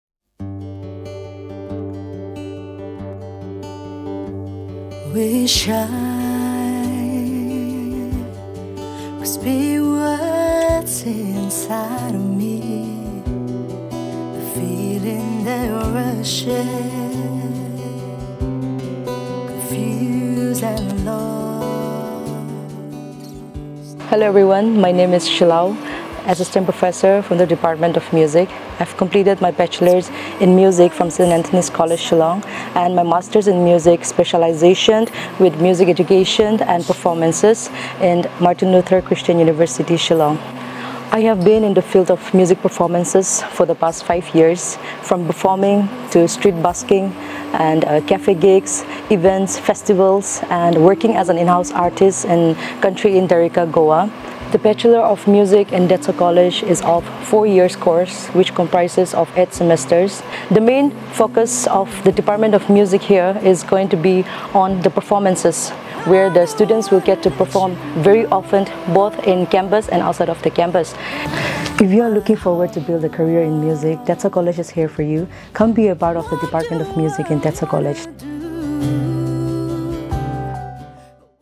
Tetso College Music Department Introduction
An introduction to one of the Music faculty in the department of music
N. Shelao.

Check out her youtube channel @ShelaoKonyakNaga